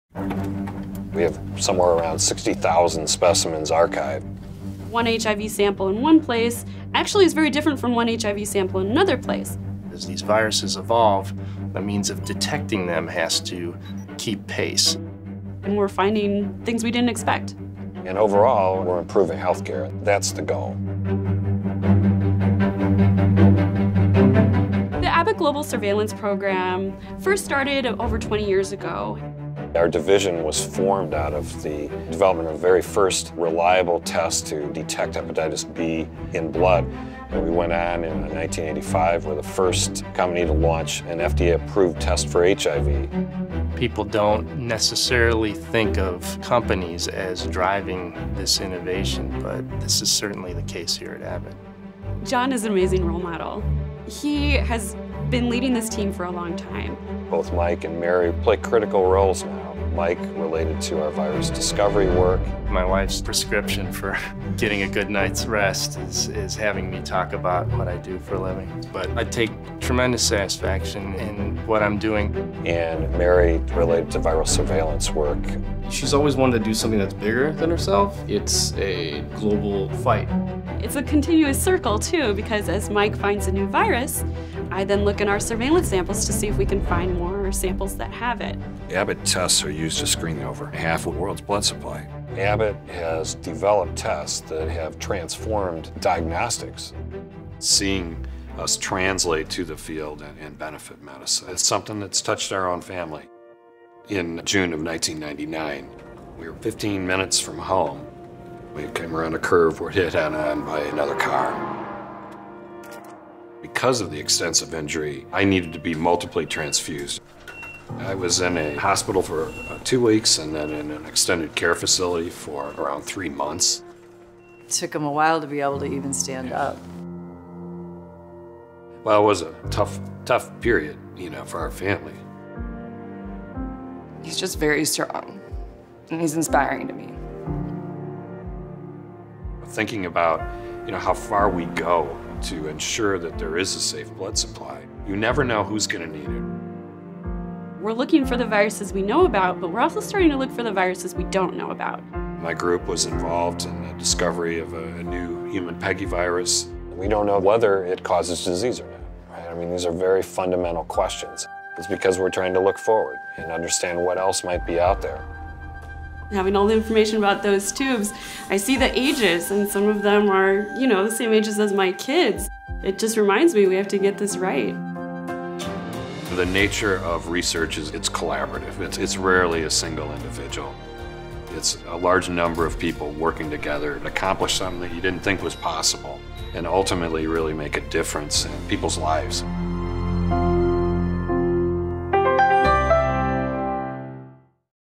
The Virus Hunters
The Virus Hunters, Abbott scientists working to identify, catalog, track and test mutating strains of HIV, hepatitis, and more, are members of Abbott’s Global Surveillance Program and new virus discovery efforts. The Surveillance Program is a one-of-a-kind collection of HIV and hepatitis virus strains from around the world, including more than 60,000 samples collected over 20 years from 40 countries on six continents.

That program in turn helps further the capabilities of many of Abbott’s diagnostics tools. Because if a virus can’t be found in testing, it can't be treated. That's how outbreaks happen.

The battle is constant. Viruses are always evolving. It's their fight for survival as much as humanity's.

The global fight begins here with Abbott and the Virus Hunters.